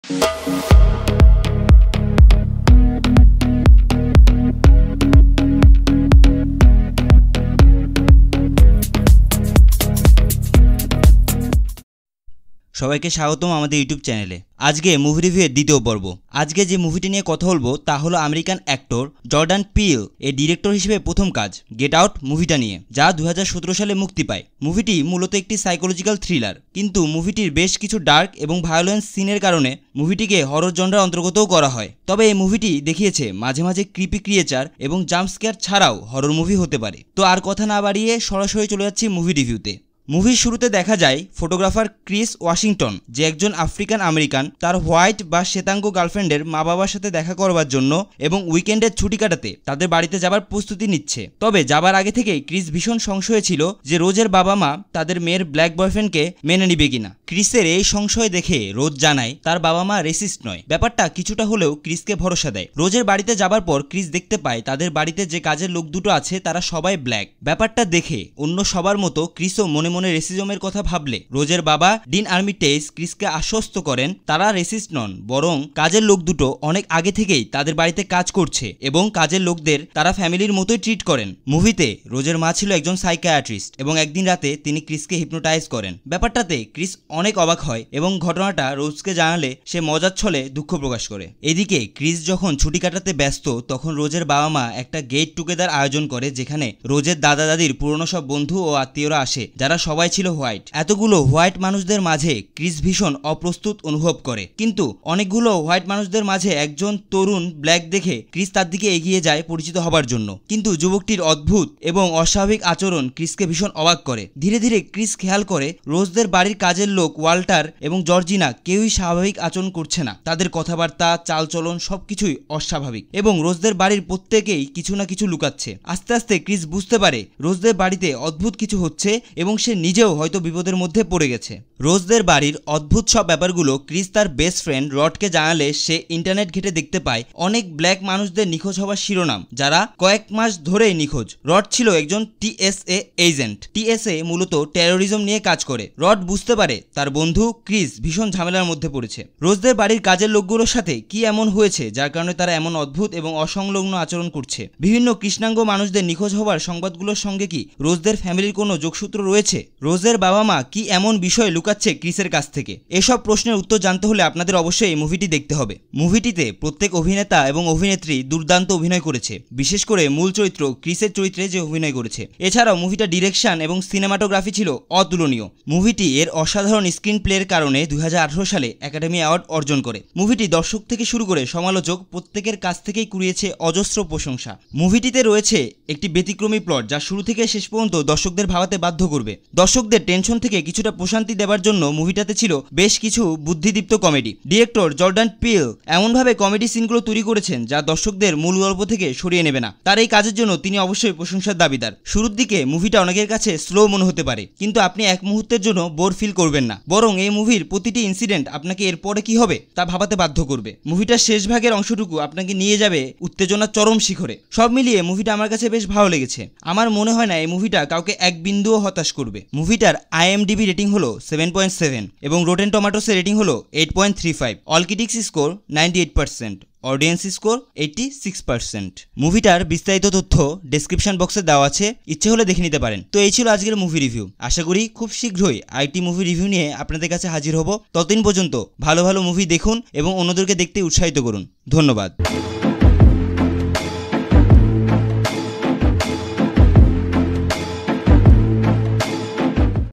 Movie Review-02 | Get Out ( 2017 ) | Jordan Peele | Ahmed & Rahman Creations ( No Spoiler )
Movie Review-02

Movie:  Get Out ( 2017 )
Genre: Horror, Mystery, Thriller
Director: Jordan Peele
Writer: Jordan Peele
IMDb Rating: 7.7/10
Rotten Tomatoes ( ALL CRITICS ) Rating: 8.35/10

Characters in Movie:

1. Daniel Kaluuya as Chris Washington
2. Allison Williams as Rose Armitage
3. Catherine Keener as Missy Armitage
4. Bradley Whitford as Dean Armitage
5. Marcus Henderson as Walter
6. Betty Gabriel as Georgina, and more.

Review and Voice: Sahed Ahmed